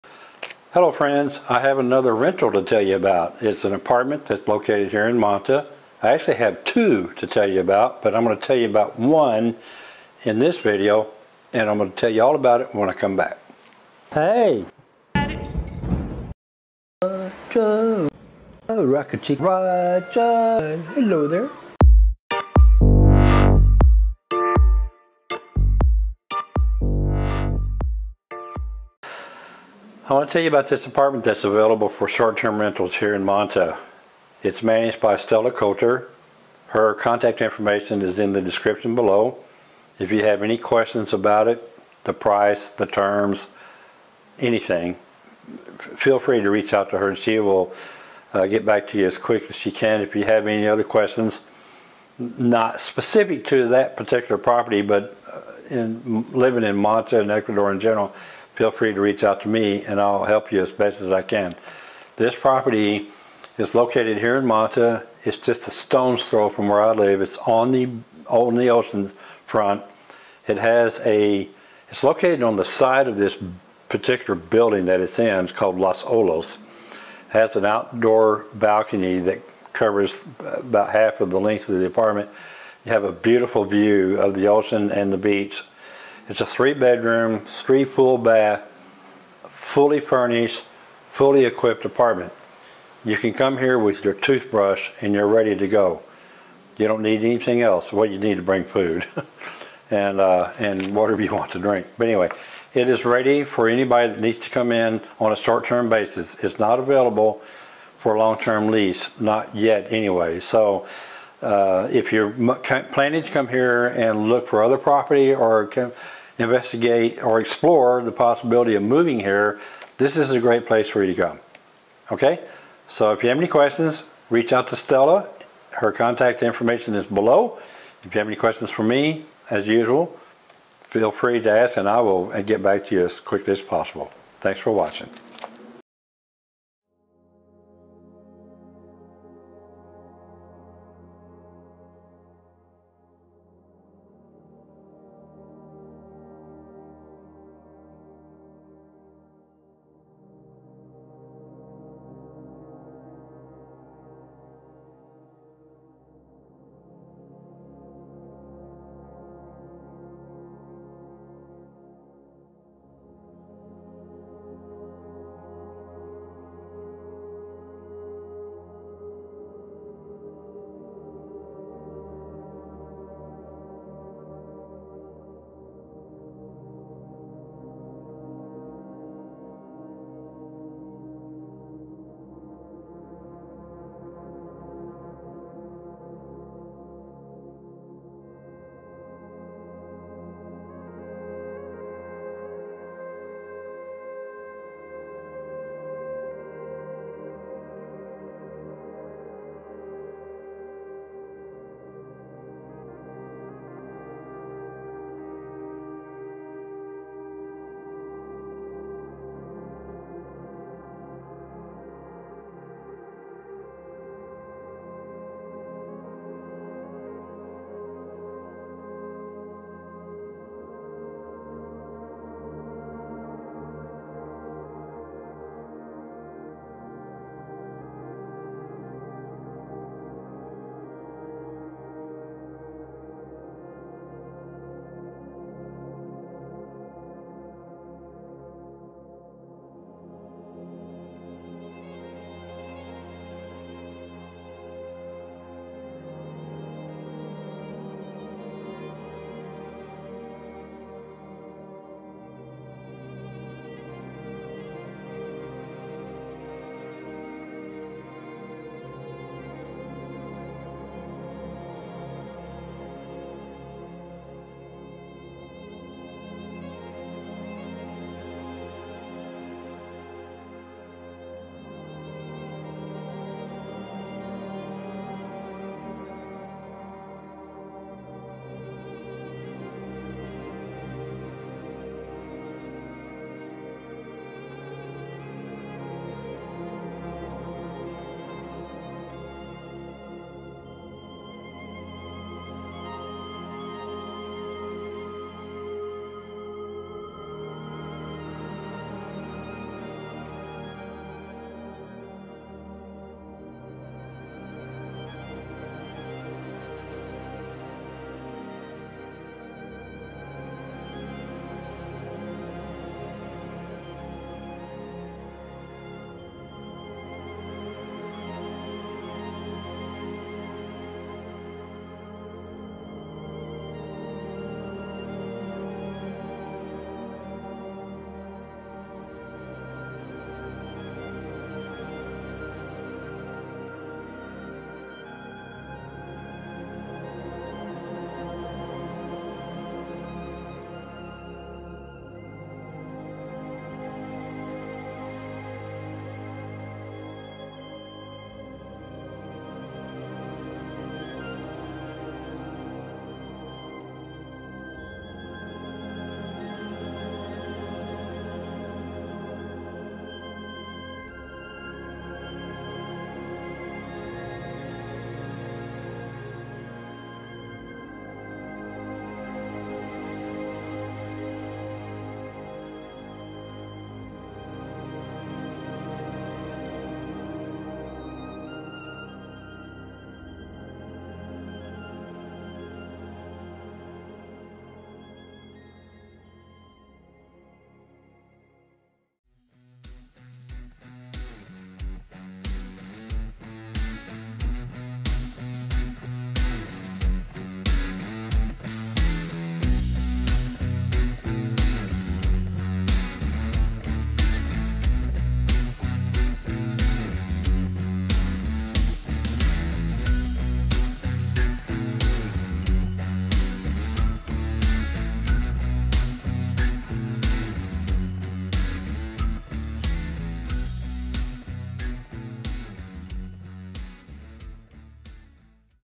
Apartments for rent or sale in Manta, Ecuador
This apartment is for rent here in Manta on a short-term rental basis. You can rent it for a month, two months, or more and it's in the perfect location for a place to stay while you e
explore Manta and other areas of Ecuador.

Music in the video courtesy of:
Scott Buckley